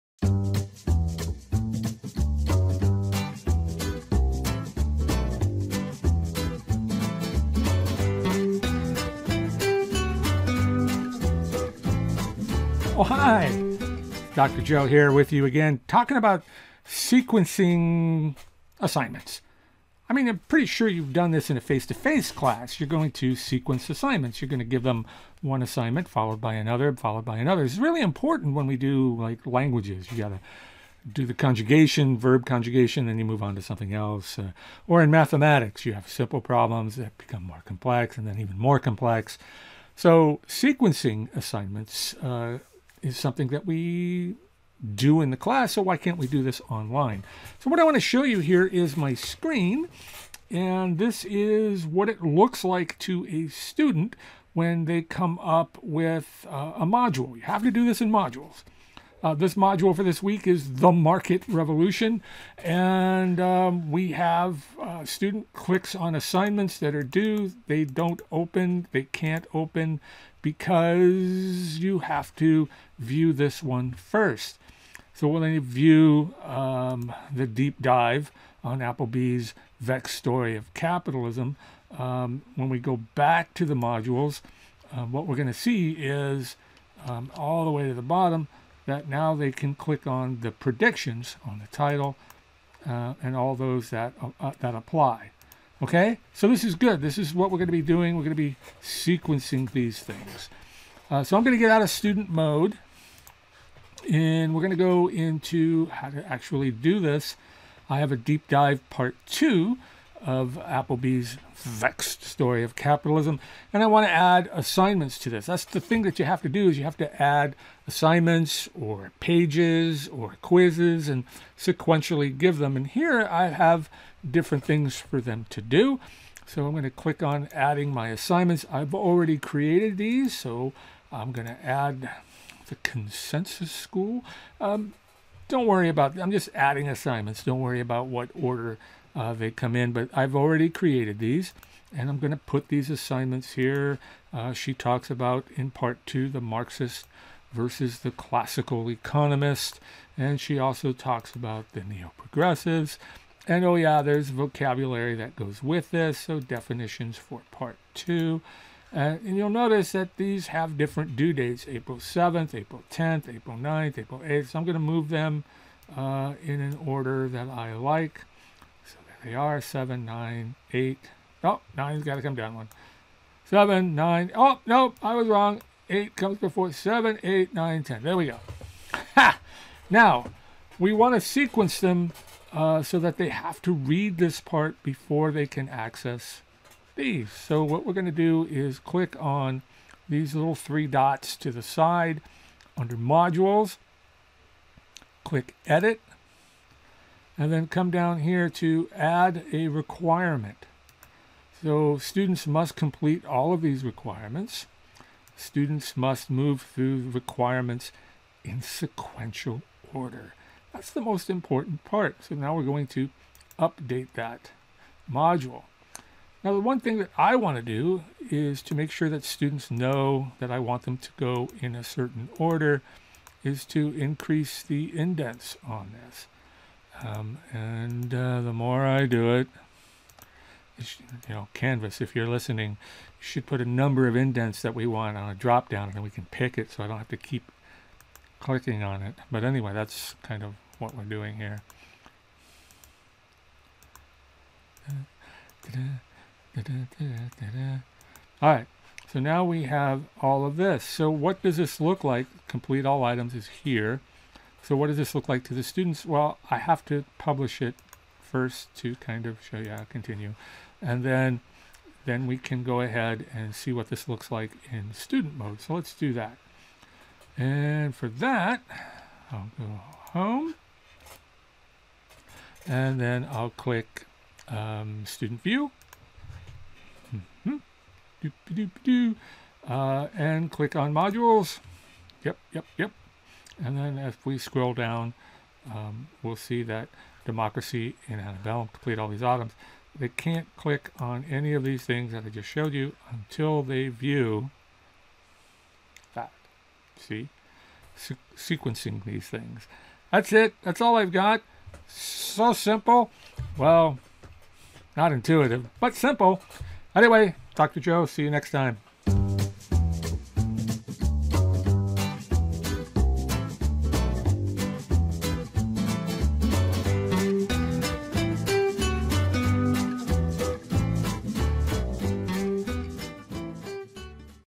How to "Sequence Assignments" using Canvas LMS
Sequencing assignments in the Canvas LMS is not intuitive, but it is simple. This video shows you how to have students read or perform assignments sequentially - just like in a face to face classrom